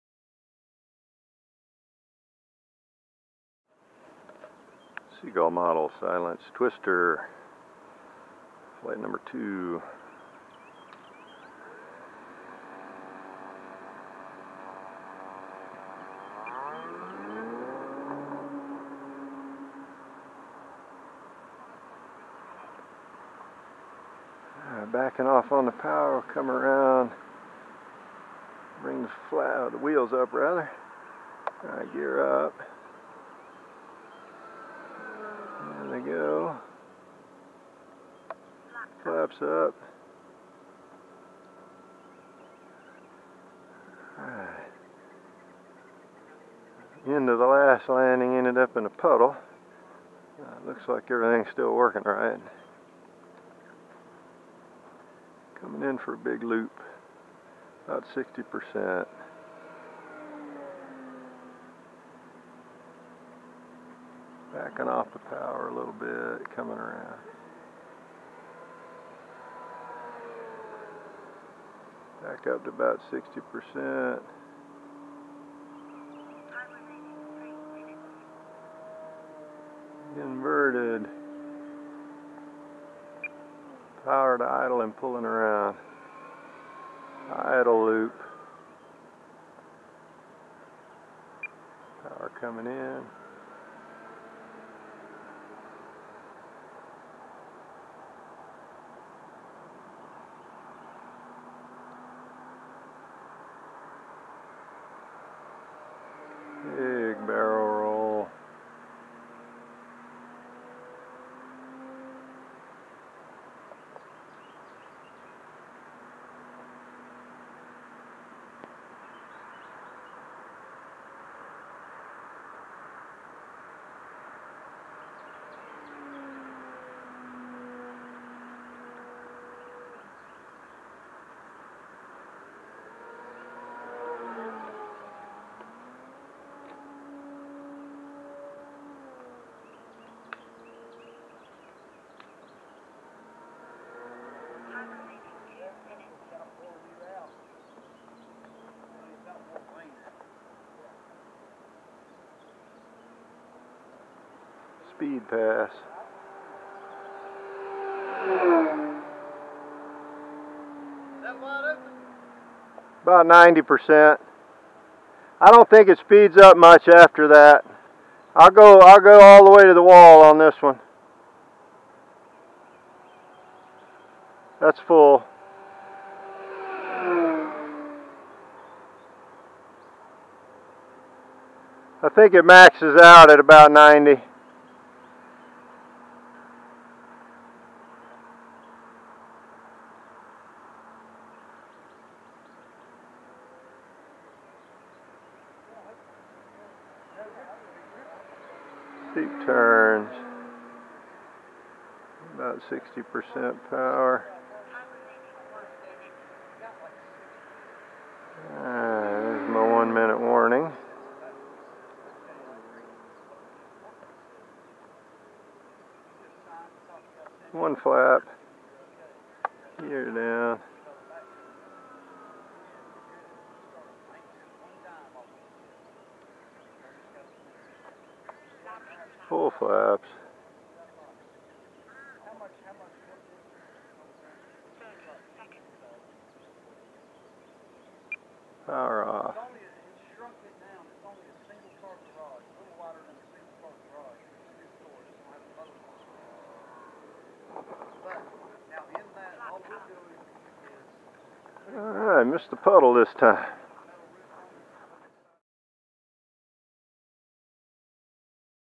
Seagull Models Silence Twister RC Plane ... Second Twist, First Day of Summer
On the second try with the Twister, I managed to miss the water.